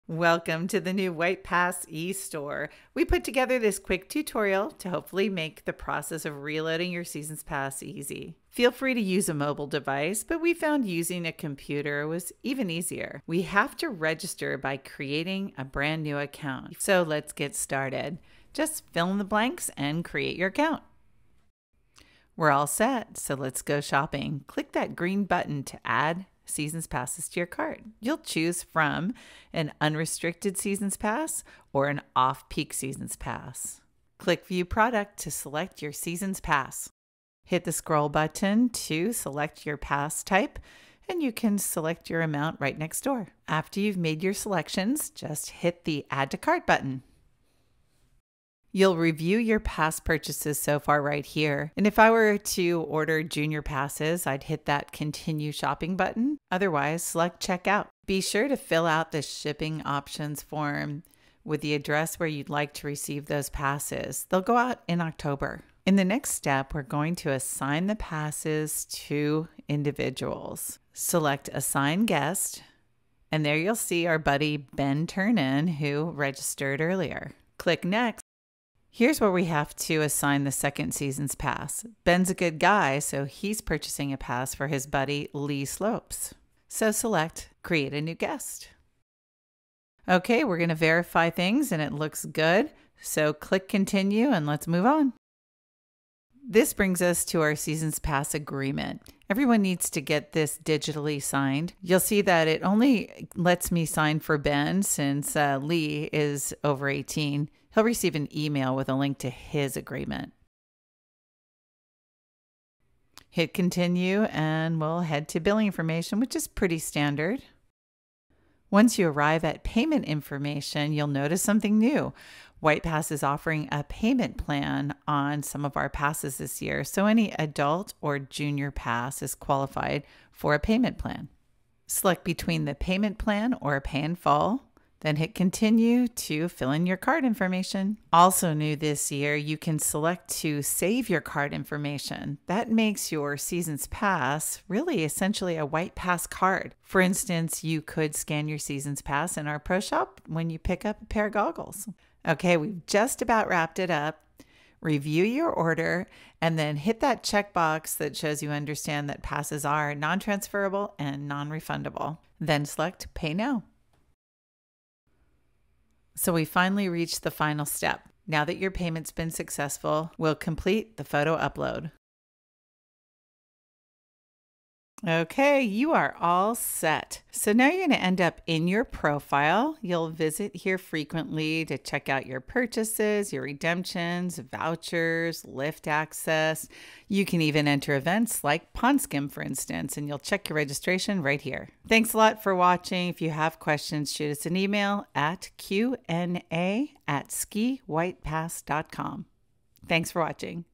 Get Familiar with the new White Pass eStore
Take a few minutes to familiarize yourself with the new WhitePass eStore. This quick tutorial will make your season pass purchases easier.